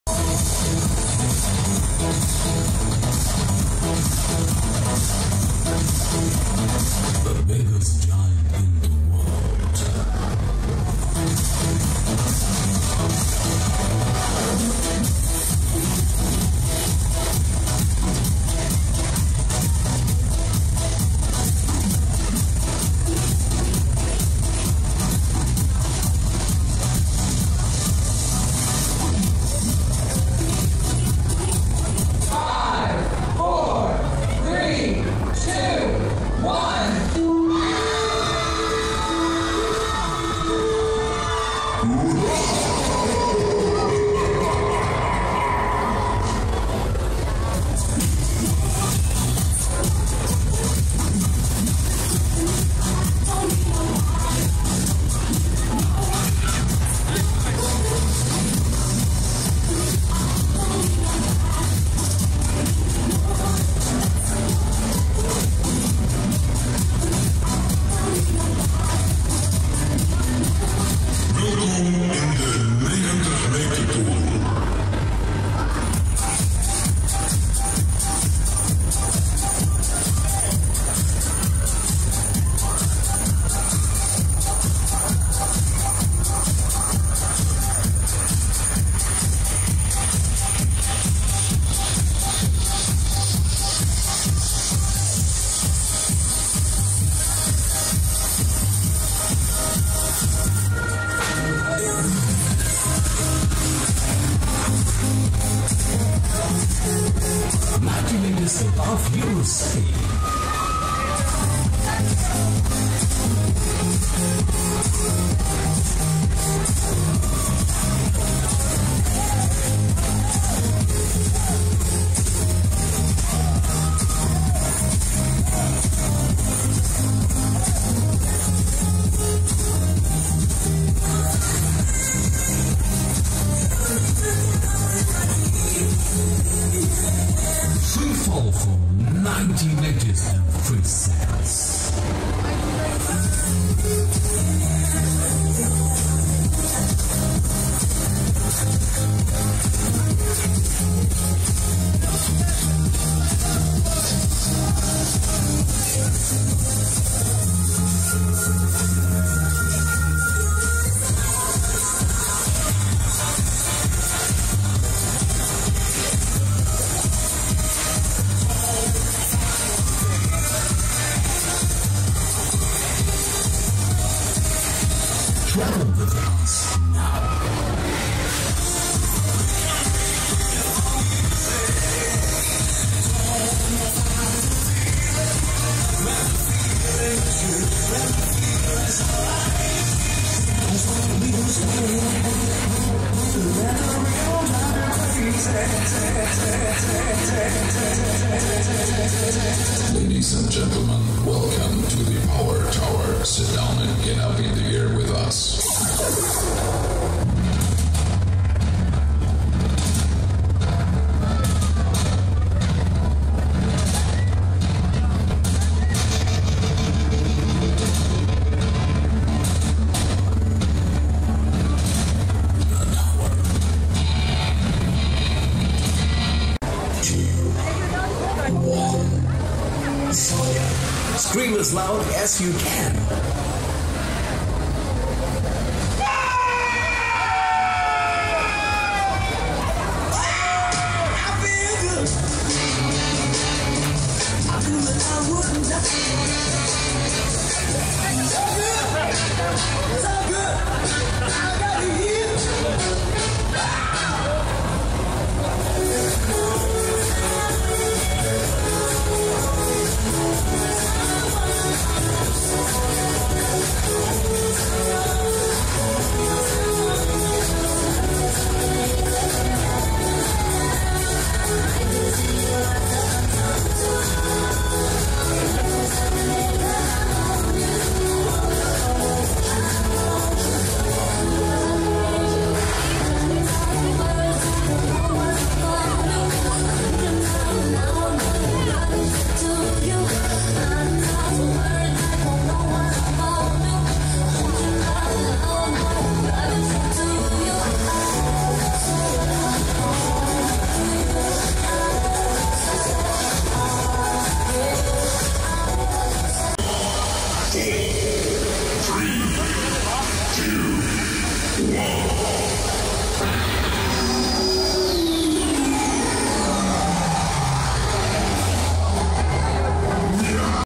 KermisTV 2015 achter de schermen - Frietje in de Tower (Free Fall)
KermisTV 2015 achter de schermen. Dit keer gaat Ron Schuurkes samen met kermiswethouder Erik de Ridder een frietje eten in de Tower (90 meter hoge vrije val).